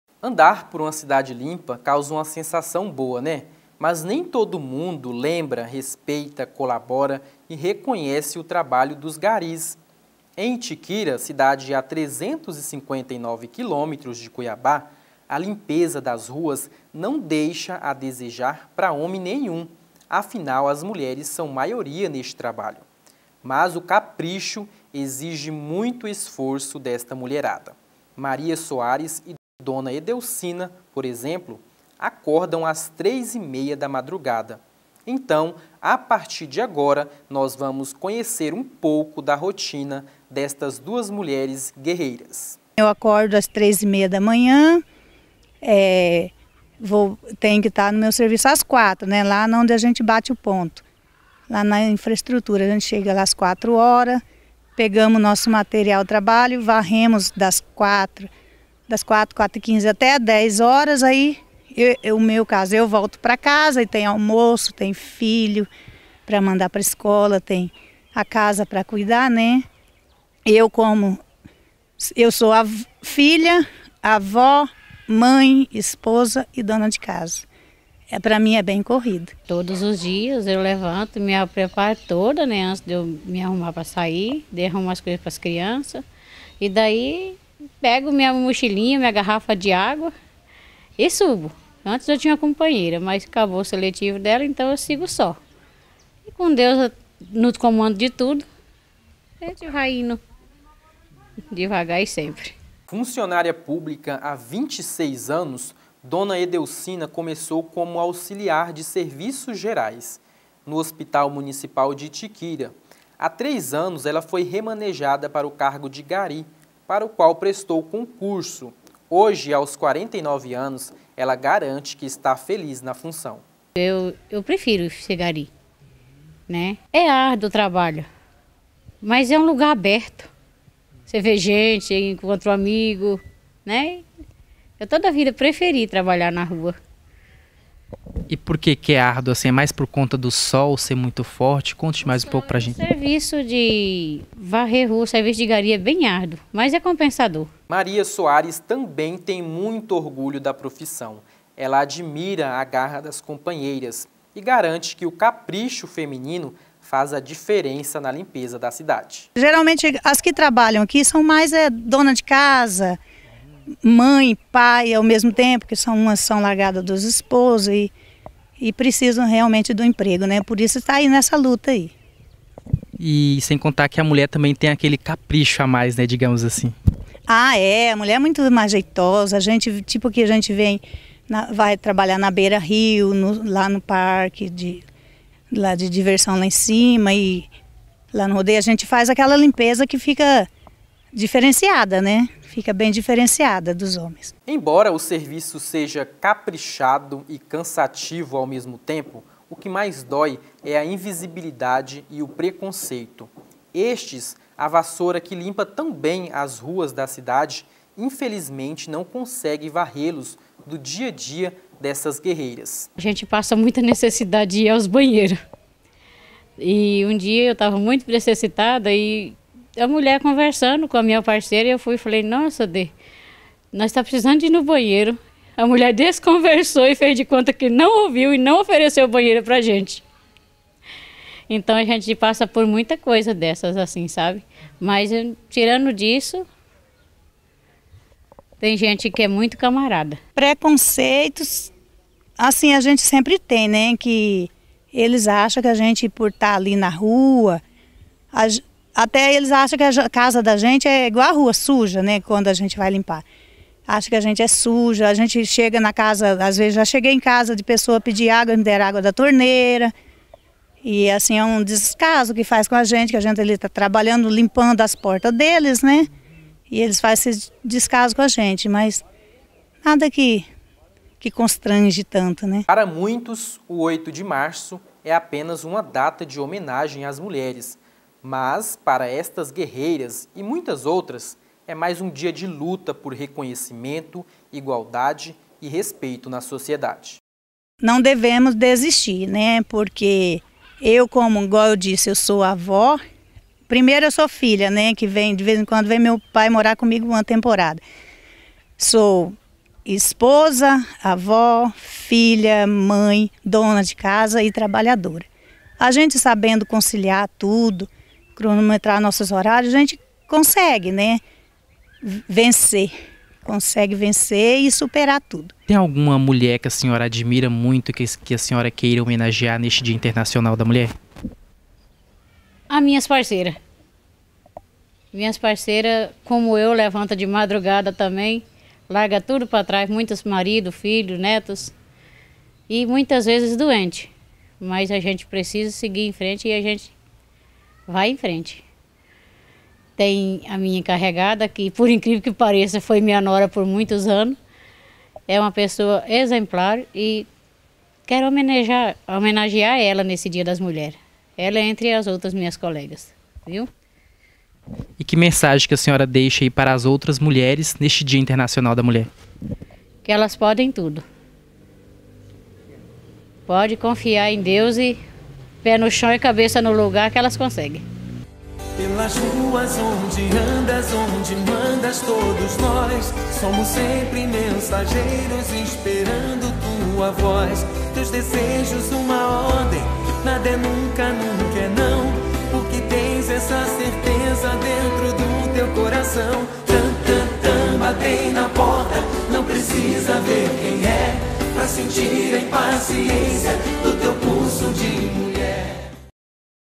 HOMENAGEM ÀS MULHERES DONA EDELCINA E MARIA SOARES FALAM SOBRE A ROTINA DE GARI
Record TV Itiquira Canal 07